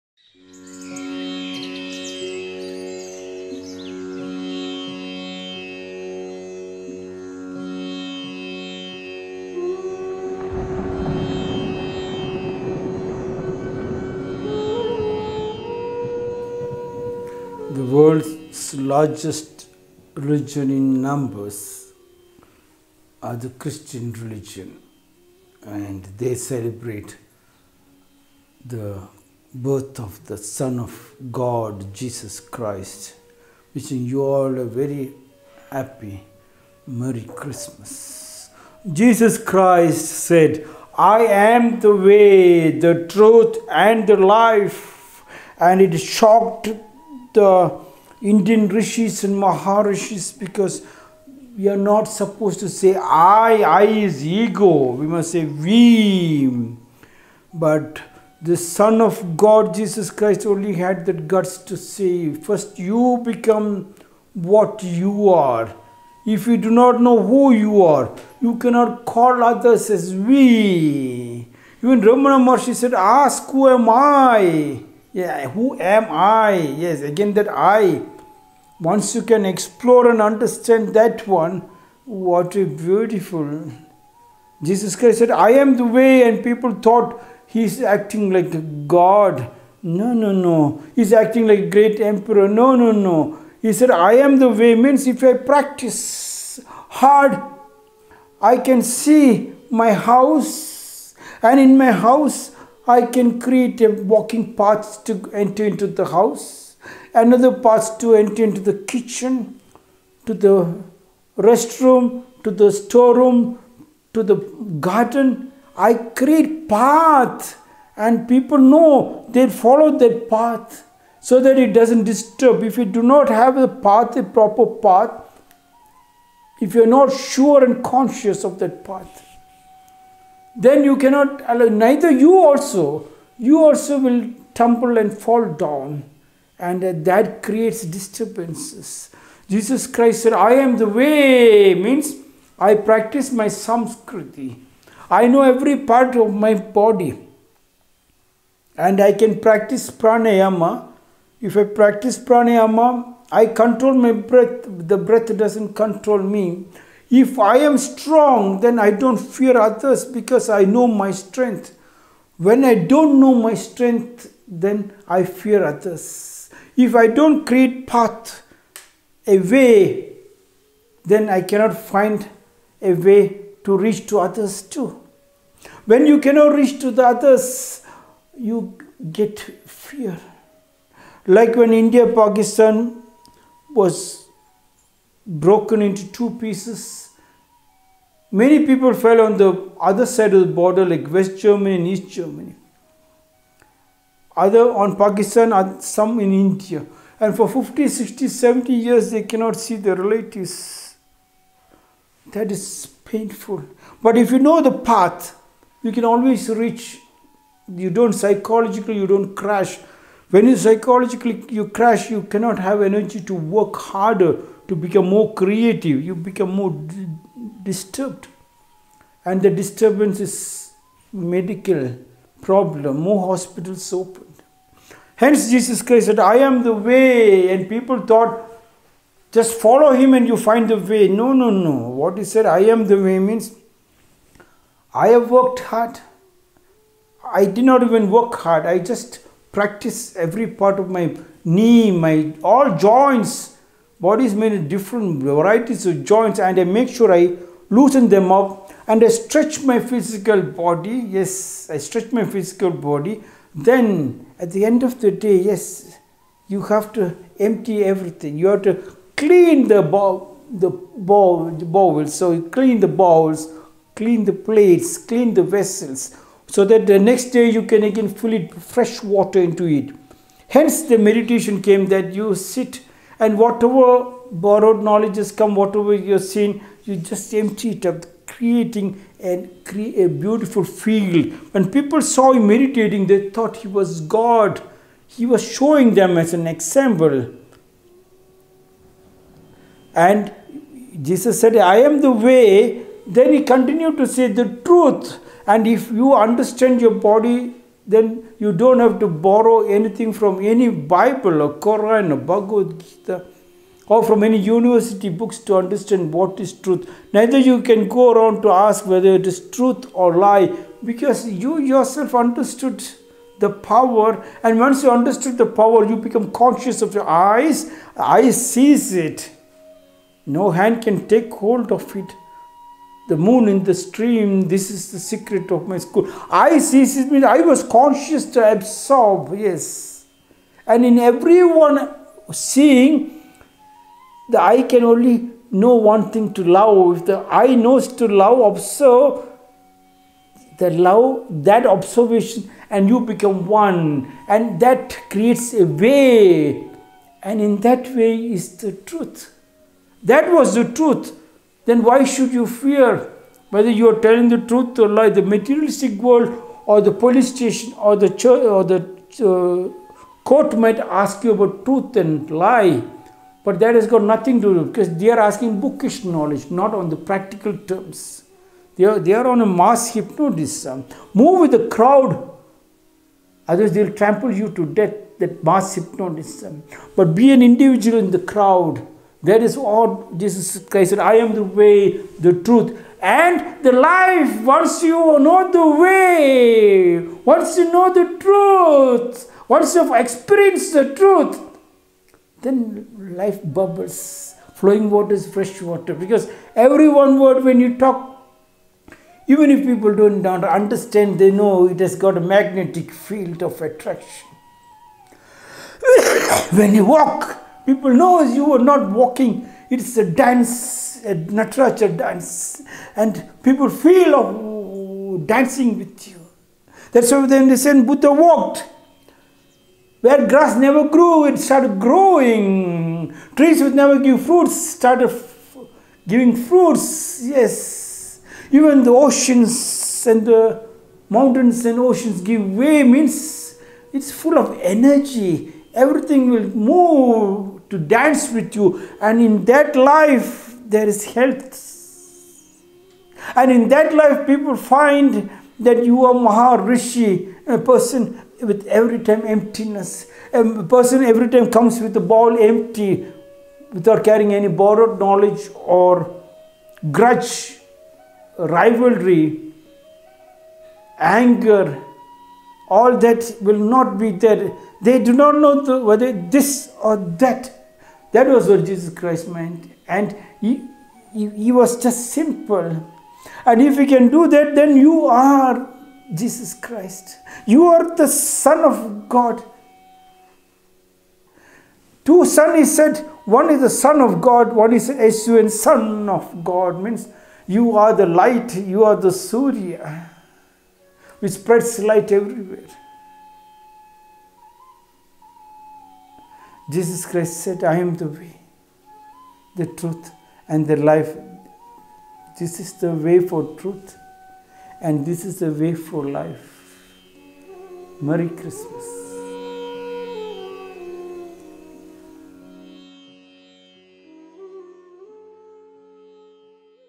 Swami Rakumjee, 987th short discourse on Jesus Christ
Jesus Christ : Iam the way , the truth, and the life."

"This is the way for truth & life."